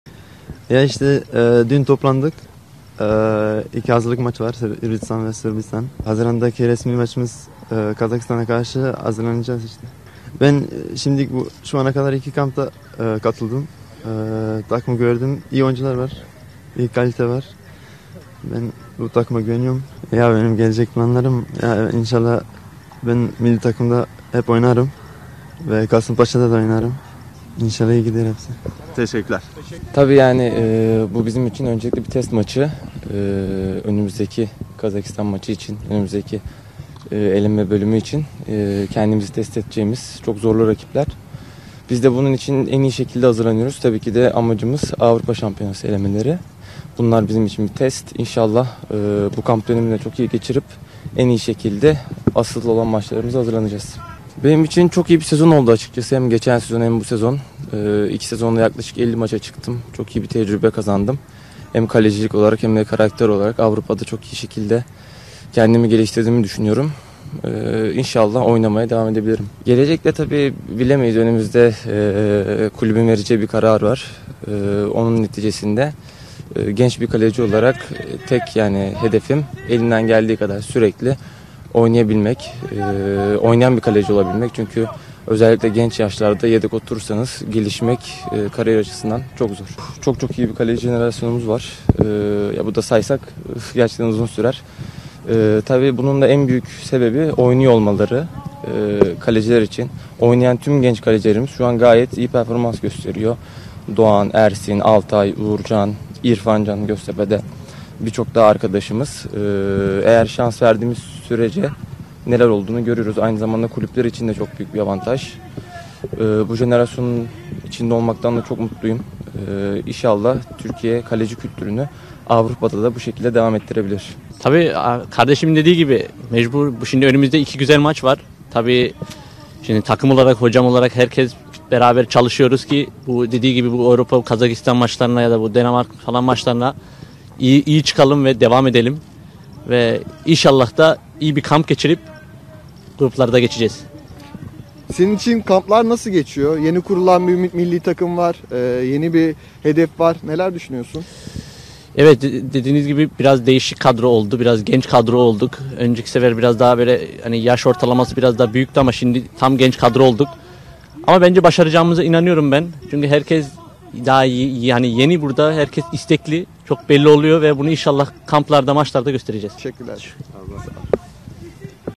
Berke Özer, Fenerbahçe'ye dönecek mi? İşte o sözler... | Westerlo
Hırvatistan ve Sırbistan hazırlık mücadeleleri öncesi İstanbul’da toplanan Ümit Milli Takım’da, Doğucan Haspolat, Berke Özer ve Melih İbrahimoğlu basınla buluştu.
Ümit Milli Futbol Takımı, haziran ayında başlayacak 2023 UEFA U21 Avrupa Şampiyonası Grup Eleme müsabakaları öncesinde, 26 Mart Cuma günü Zagreb’te Hırvatistan ve 30 Mart Salı günü Alanya’da Sırbistan U21 Milli Takımları ile oynayacağı özel maçların hazırlıklarına İstanbul’da başladı. Kasımpaşa Kemerburgaz Tesislerinde yapılan idman öncesi, Doğucan Haspolat, Berke Özer ve Melih İbrahimoğlu, basın mensuplarına açıklamalarda bulundu. Kasımpaşa’da forma giyen Doğucan Haspolat, “Dün toplandık. İki hazırlık maçımız var Hırvatistan ve Sırbistan. Haziran’da oynayacağımız Kazakistan resmi maçına hazırlanıyoruz. Şu ana kadar iki kampa katıldım. Takımda iyi oyuncular var. Kaliteli bir takımız. Takıma da güveniyorum. İnşallah Avrupa şampiyonasına gideriz. İnşallah milli takımda ve aynı zamanda da Kasımpaşa’da hep forma giyerim. Her şey umarım benim için iyi olur” dedi.

Berke Özer: “Avrupa’da kendime çok şey kattım”
Belçika ekibi Westerlo’da mücadele eden Berke Özer ise iyi bir kamp dönemi geçirmek istediklerini belirterek, “Şu an önümüzde iki test maçımız var önümüzdeki eleme maçları için. Kendimiz test edeceğimiz zorlu rakipler. Amacımız, Avrupa şampiyonası elemeleri. Bu kamp dönemini iyi geçirmek istiyoruz” dedi. Avrupa’da iyi bir kariyer geçirdiğini ifade eden Özer, “Son iki sezon benim için iyi geçti. 50’ye yakın maça çıktım. Hem kalecilik hem de karakter olarak Avrupa’da kendimi iyi geliştirdim. Önümüzde kulübümüz vereceği bir karar var. Hedefim, elimden geldiği kadar oynamak. Oynamak ok önemli. Özellikle genç kaleciler için. Ülkede çok iyi kaleci jenerasyonu var. Bunun da en büyük sebebi oynamaları. Oynayan tüm genç kalecileri iyi performans sergiliyor. Doğan, Ersin, Altay, Uğurcan, İrfancan gibi. Eğer şans verilirse neler olduğunu görülüyor. Umarım Türkiye kaleci kültürünü Avrupa’da da devam ettirebiliriz. Bu jenerasyonda olduğum için çok mutluyum” ifadelerini kullandı.

Melih İbrahimoğlu: “Takımda herkes çok istekli”
Heracles Almelo’da forma giyen Melih İbrahimoğlu da, “Önümüzde iki maçımız var. Takım olarak Avrupa elemelerine iyi hazırlanmak istiyoruz. Hedefimiz iyi kamp dönemi geçirerek, elemelerde iyi performans sergilemek. Şimdi genç bir kadro olduk. Başaracağımıza inanıyorum. Herkes çok istekli. Bunu maçlarda ve kamplarda göstereceğiz” şeklinde konuştu.